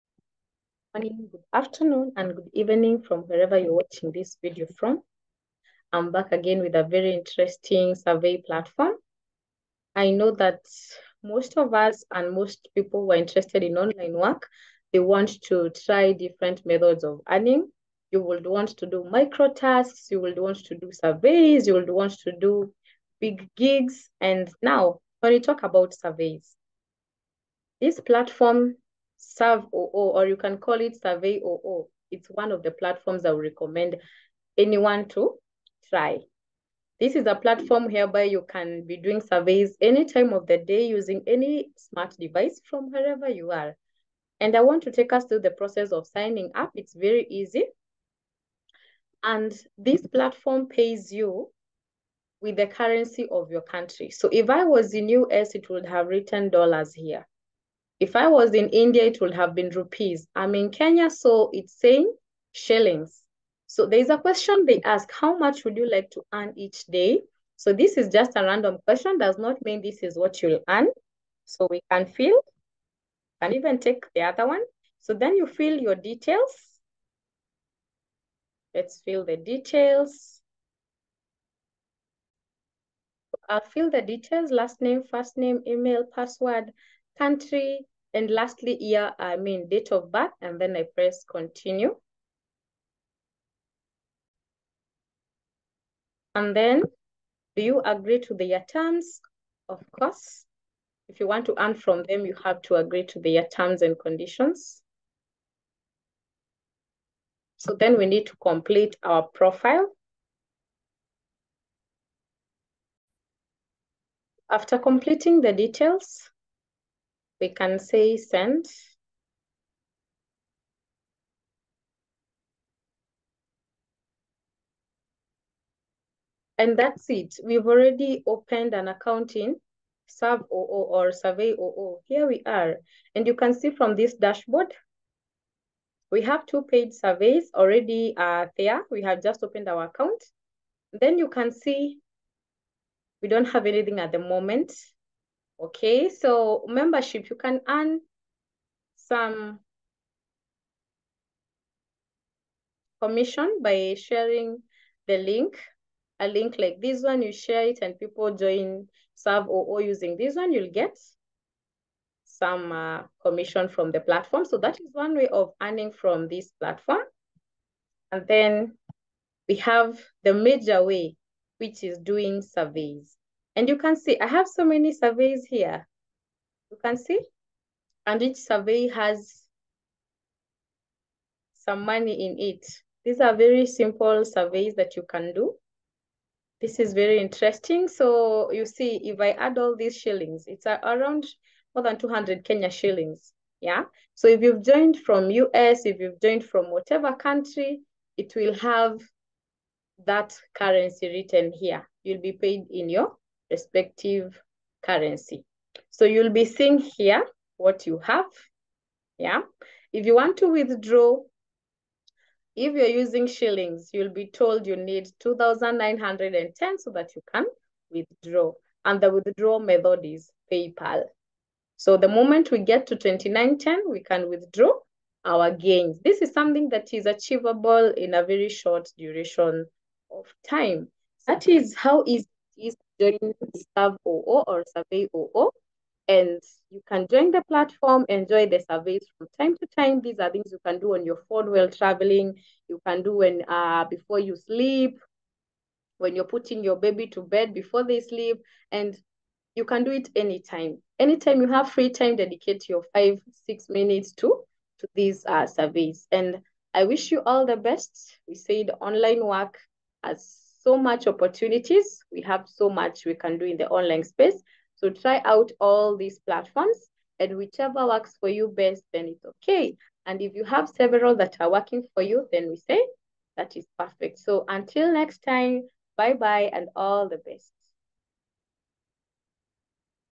🌟SURVEOO Sensation: Effortless Surveys, Instant Rewards!🌟 Mastering Surveys 101:
Surveoo is a SURVEY platform that allows freelancers to do multiple surveys. The amount you can earn from the platform is not limited. You are the one to push yourself till you harvest a lot of money by doing many surveys.